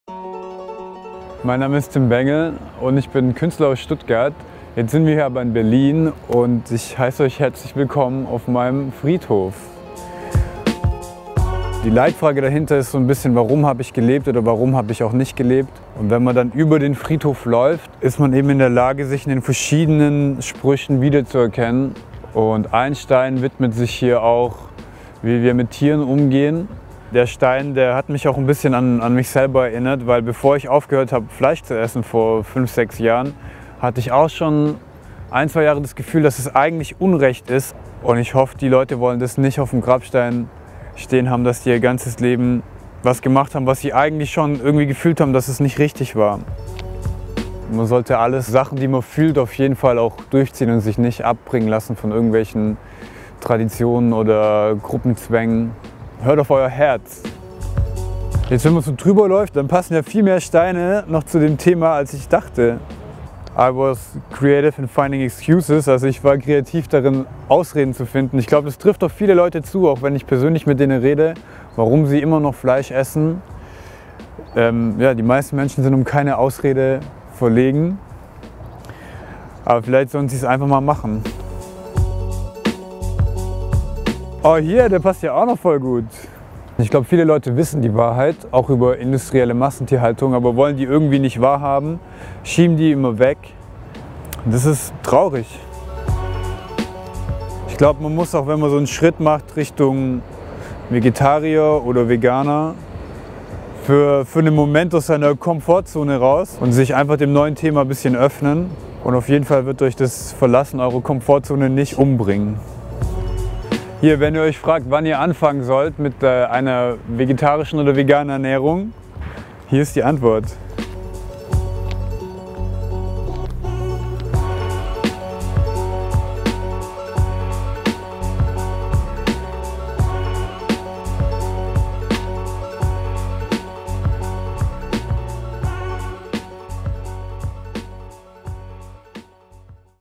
Tim Bengel – ein Grabstein für Veganer
Der Stuttgarter Ausnahmekünstler Tim Bengel, der in einer New Yorker Galerie ausstellt und auch auf der diesjährigen “Art Miami” dabei ist, überreichte uns von PETA Deutschland zu unserem 25-jährigen Jubiläum ein ganz besonderes Geschenk: Aus seiner aktuellen Kunstinstallation „Graves of our Generation“ schenkte der Veganer uns einen Grabstein aus Marmor, auf dem mit Echtgold geschrieben steht „Somehow I felt killing animals is wrong“.

Jetzt vegan werden!